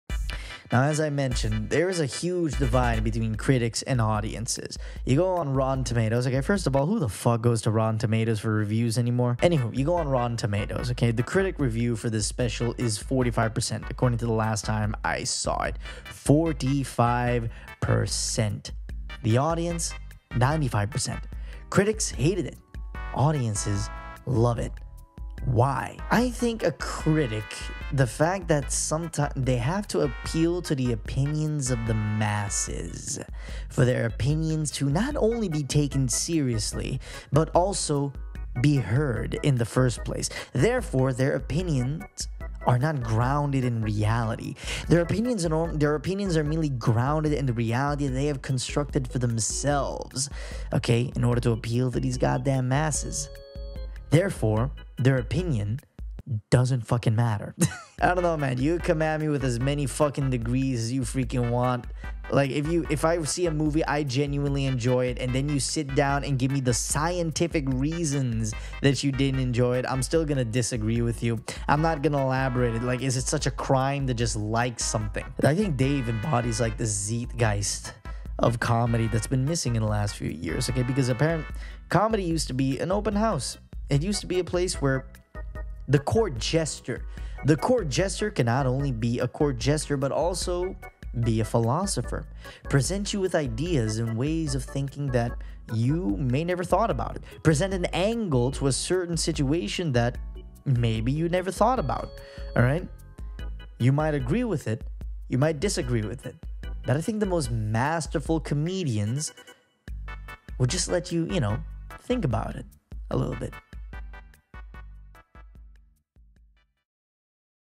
Critics vs Audience: Dave Chappelle’s The Closer (Podcast Highlight)
In my latest podcast, i went in depth about my feelings toward the controversy surrounding Dave Chappelle’s “The Closer”. Even though i think that this special is a breath of fresh air in this world that seems so suffocated by political correctness. This is a highlight of that podcast, which you can watch